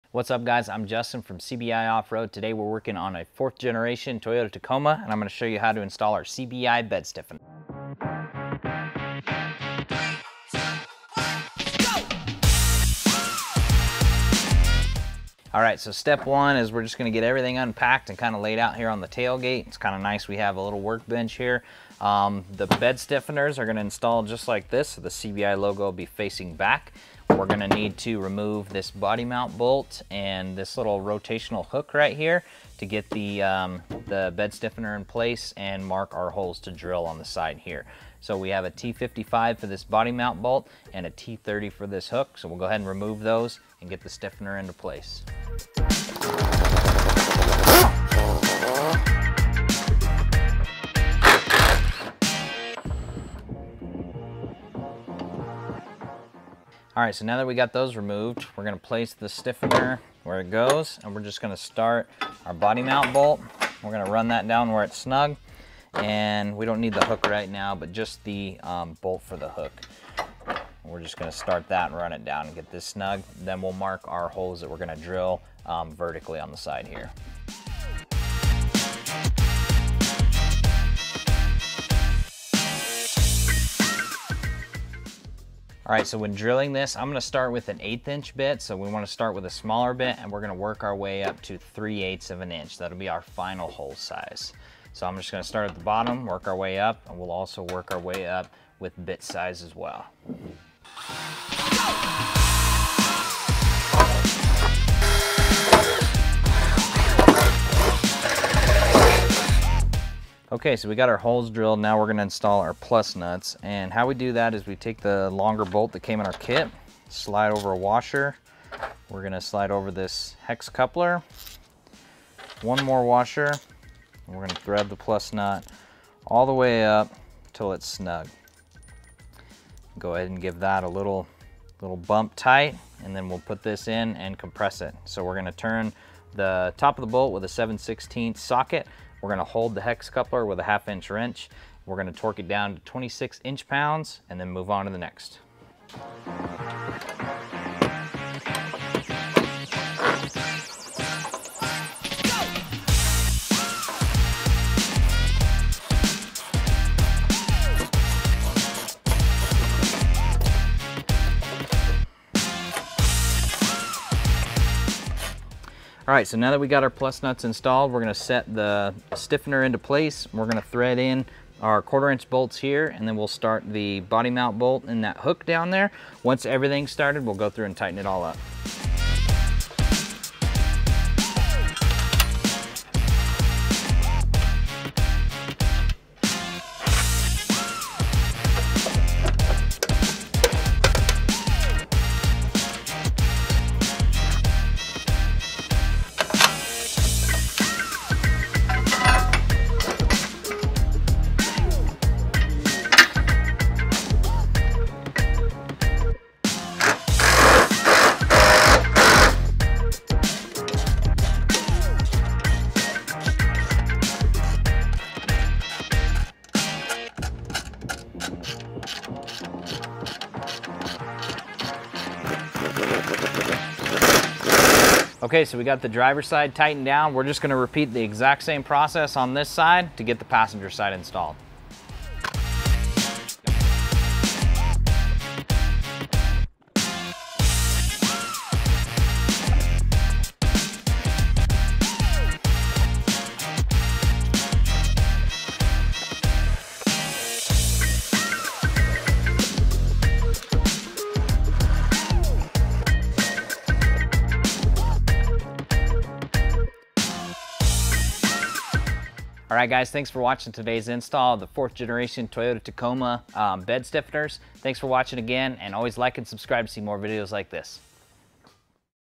4th Gen Tacoma CBI Bed Stiffeners | 2024
The CBI Bed Stiffeners are meant to give extra support to the sides of your truck bed as you start to add more weight. Prevent damages before they happen and gain some peace of mind with this product! They are designed to do more like mounting a whip or strap down points for when you need to secure what you're hauling.

Follow along with this video for step by step instructions on how to get this installed.